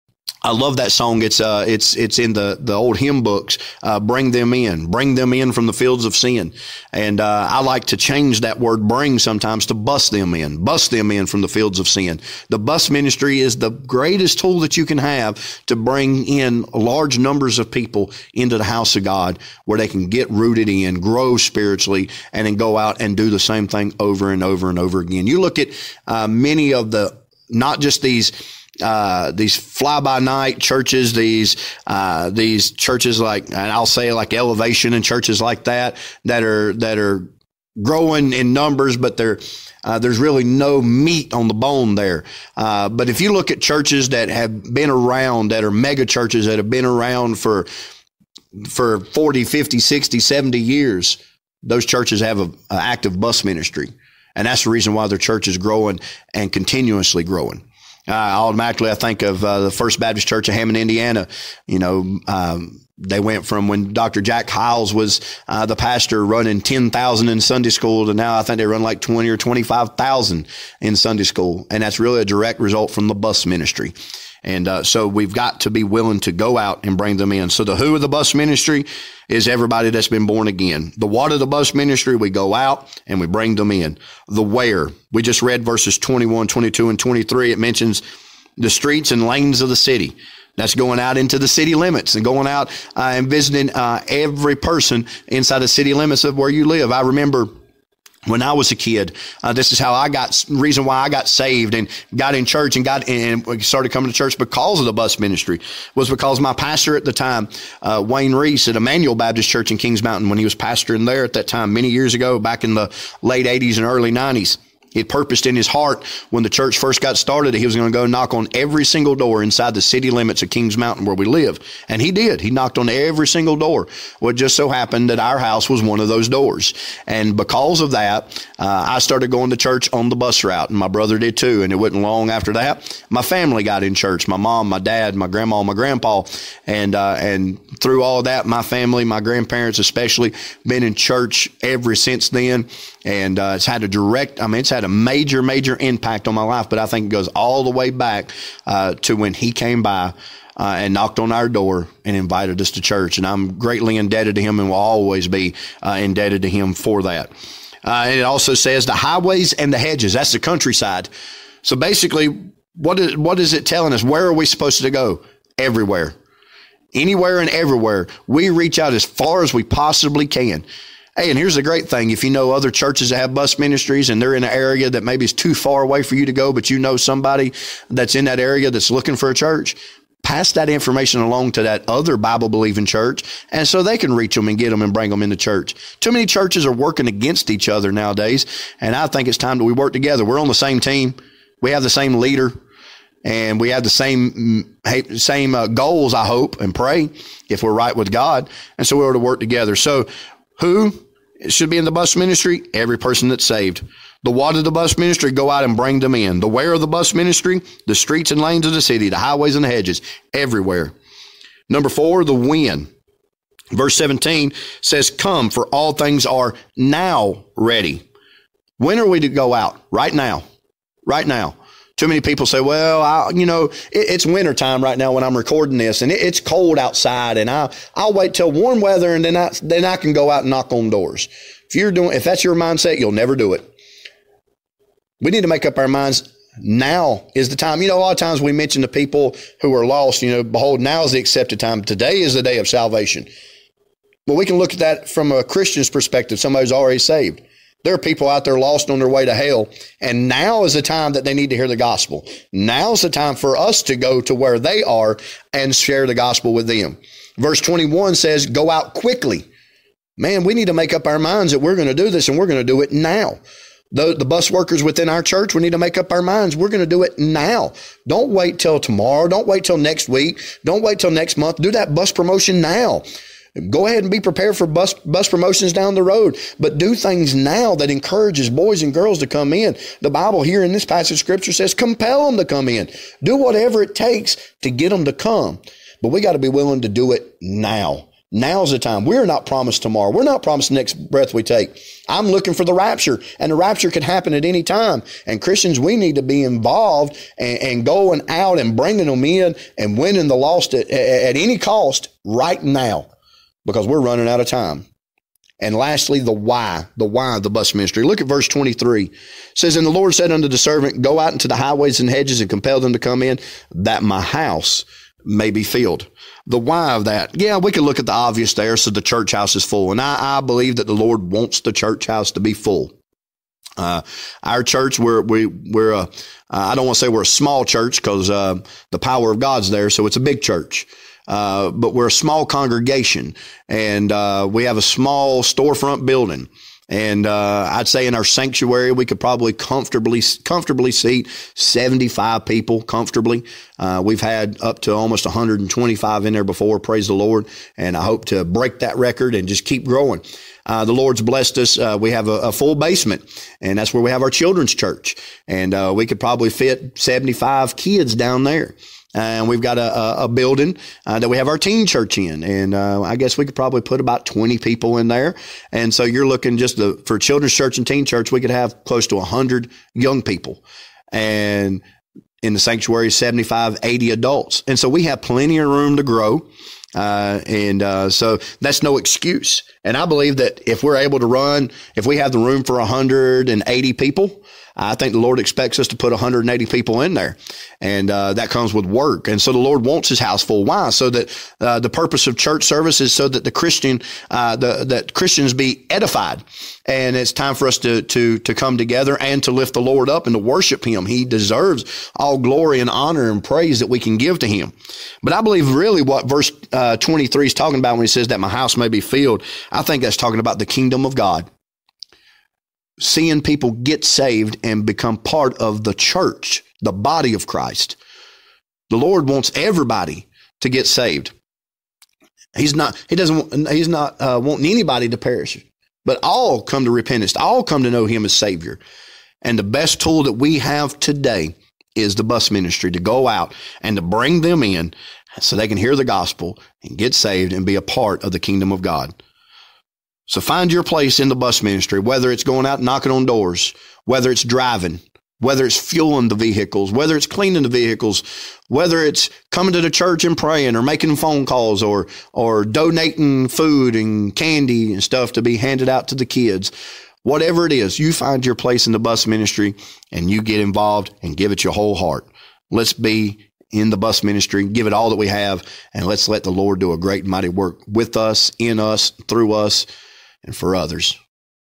Bus Ministry Nuggets - Ep 3 - Five W's of The Bus Ministry - Part 2
BMN is a weekly series focused on edifying the saints on the importance of the bus ministry by pastor Michael Rote.